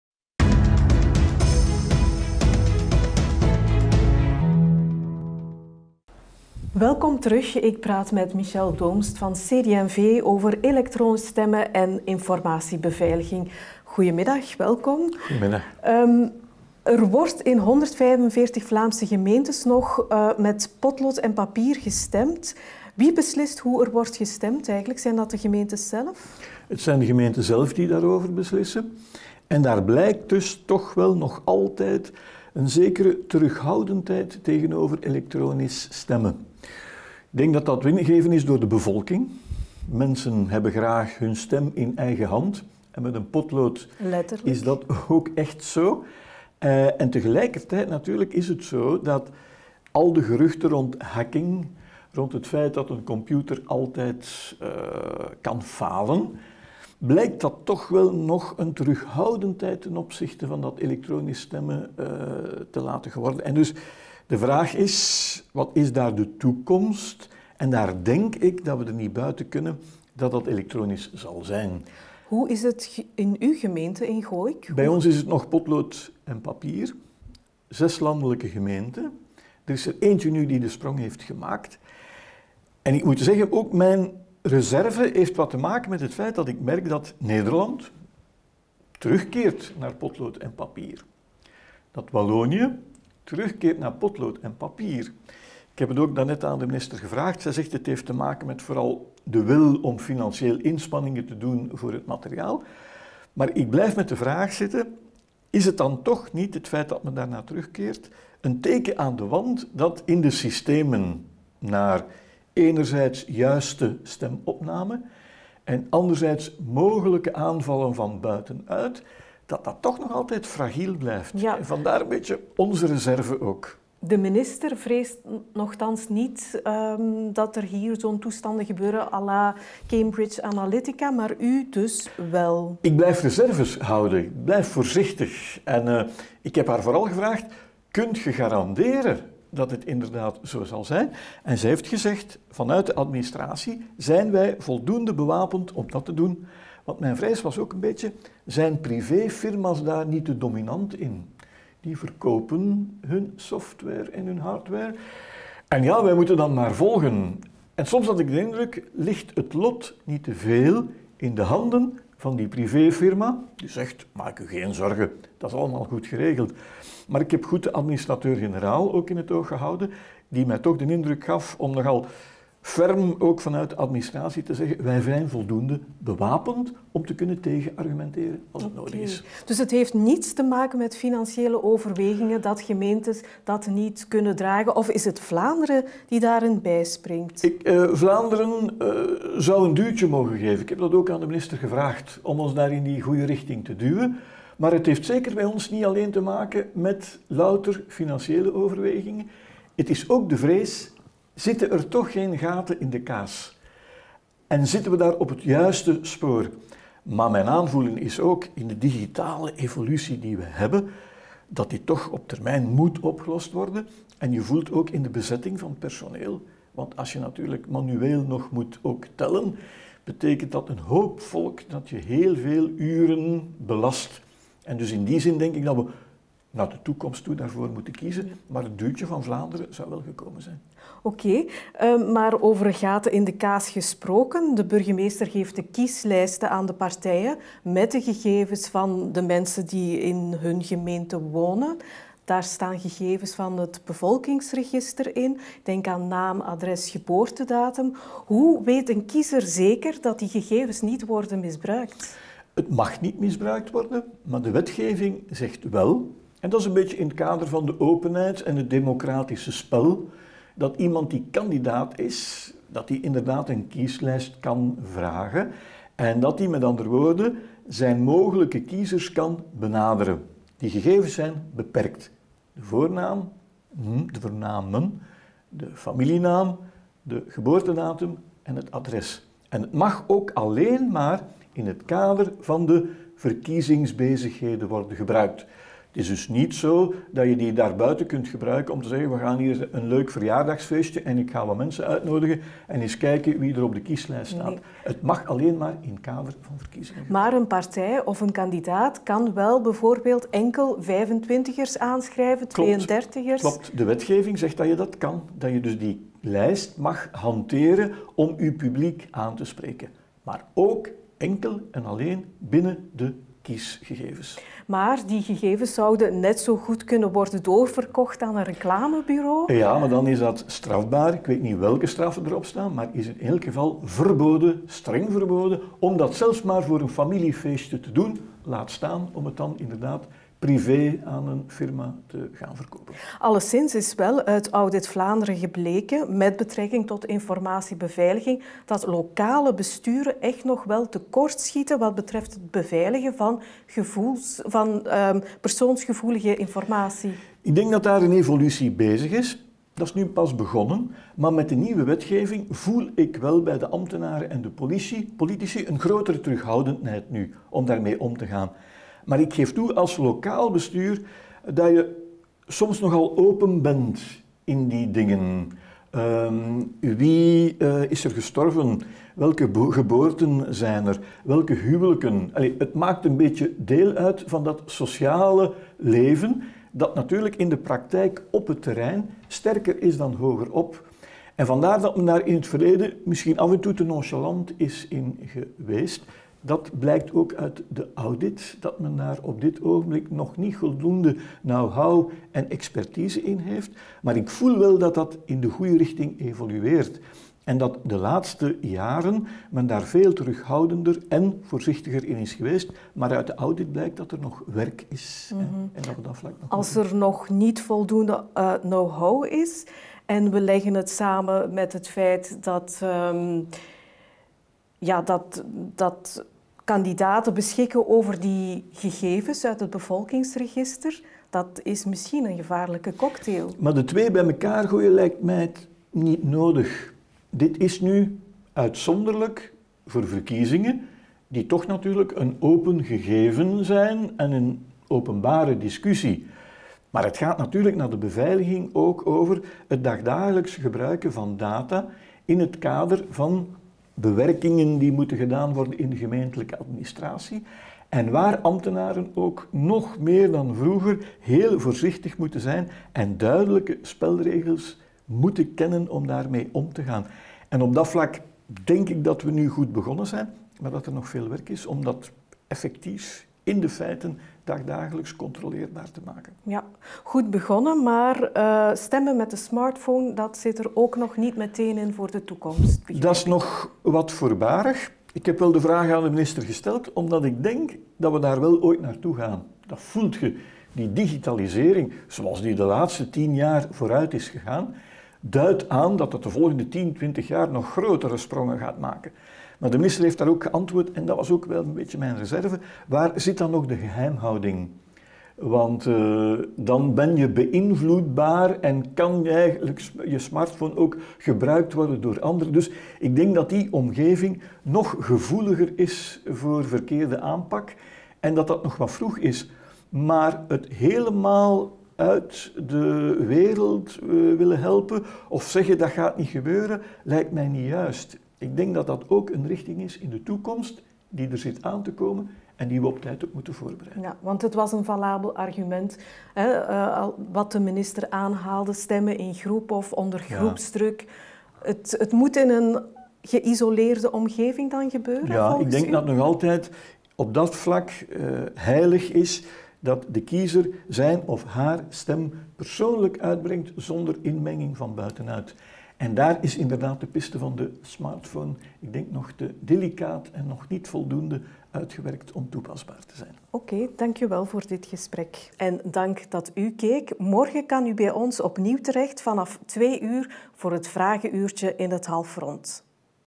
Studio Commissie: Michel Doomst over elektronisch stemmen
In het Vlaamse Gewest wordt reeds verschillende jaren ingezet op het elektronisch stemmen. Maar toch zal er bij de lokale verkiezingen van 14 oktober 2018 nog in 145 Vlaamse gemeenten gestemd worden met potlood en papier. “Nederland gaat terug naar het papier, en ook Wallonië gaat terug naar het potlood. Er blijkt dus toch nog altijd iets loos te zijn in de systematiek. Wat we horen, is dat er nog altijd gaten in de stemkaas blijven”, klinkt het bij Michel Doomst (CD&V).